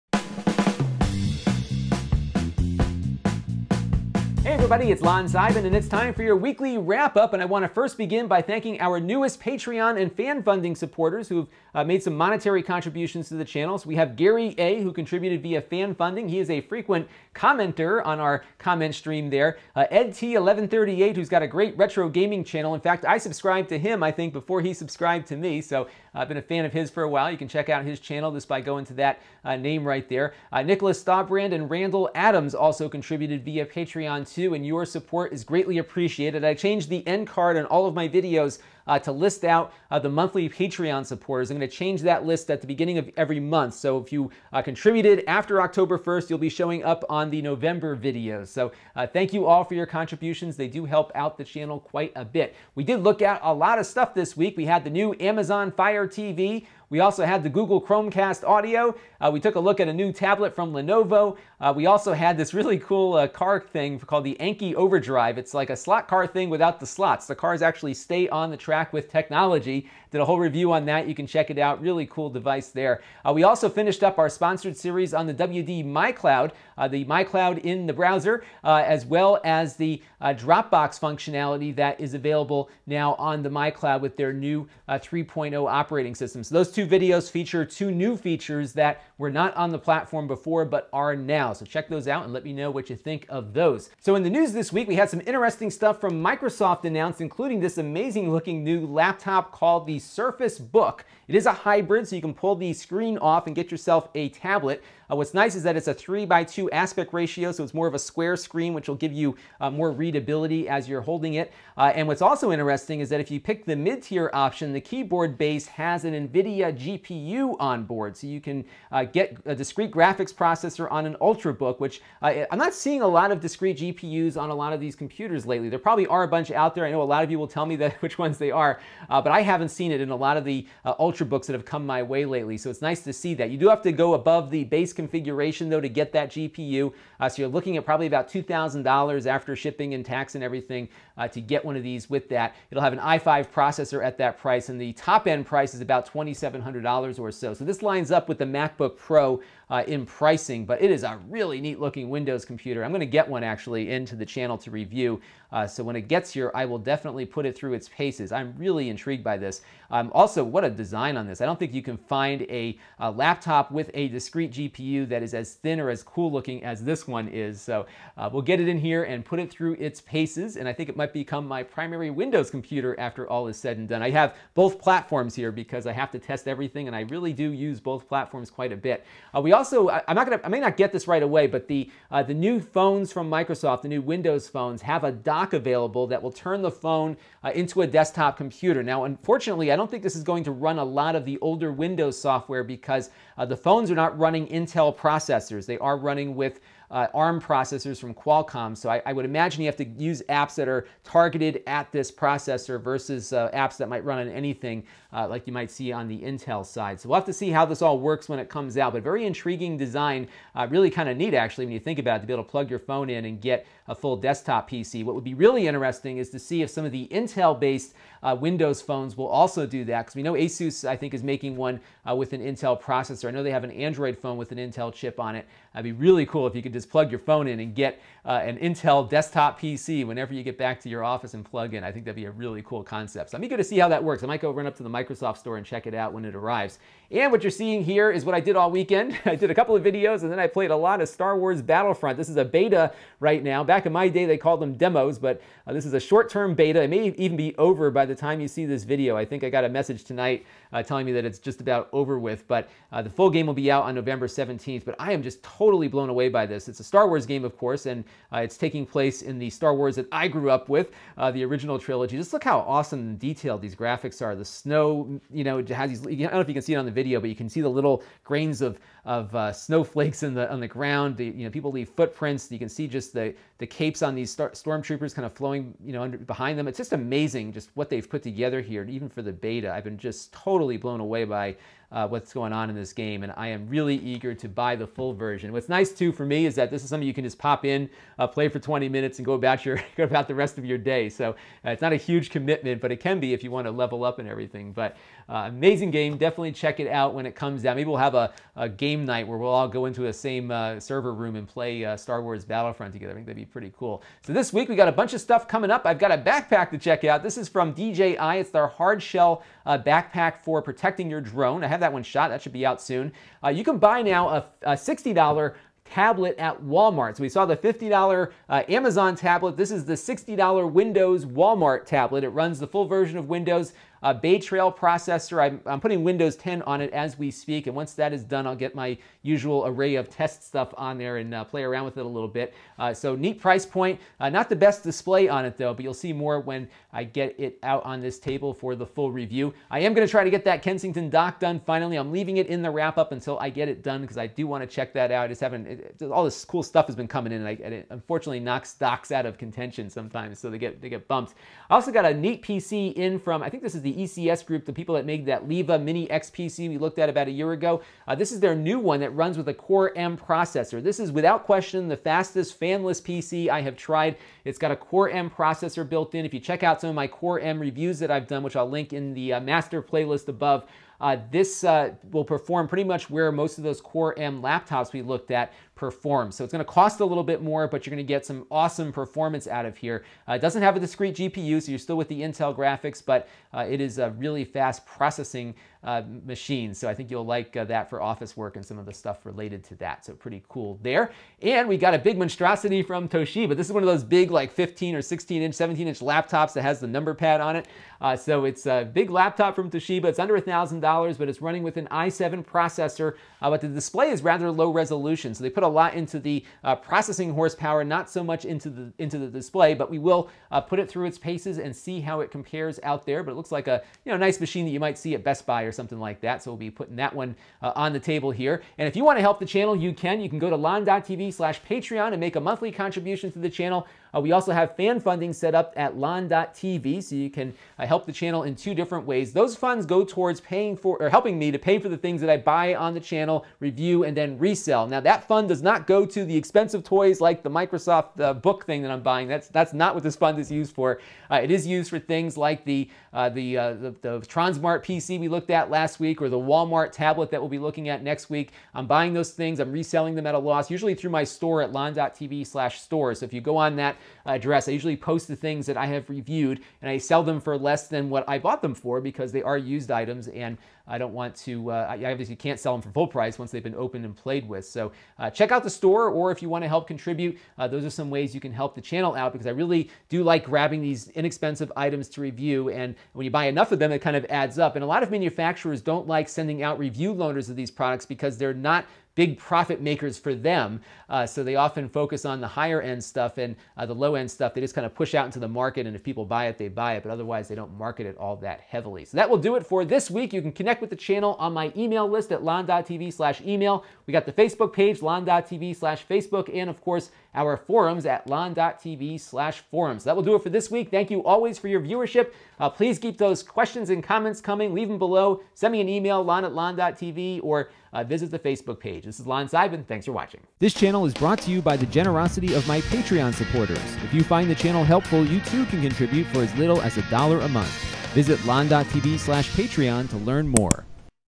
Weekly Wrapup 69 - Microsoft's new Surface Book Laptop, Star Wars Battlefront, and More
This week I talk about the interesting new products from Microsoft including their Surface Book laptop and a new dock that turns their Windows phones into desktop PCs.

I'm enjoying the new Star Wars Battlefront Beta and have a whole bunch of stuff coming up to review this week when I'm not trying to get the Luke Skywalker powerup.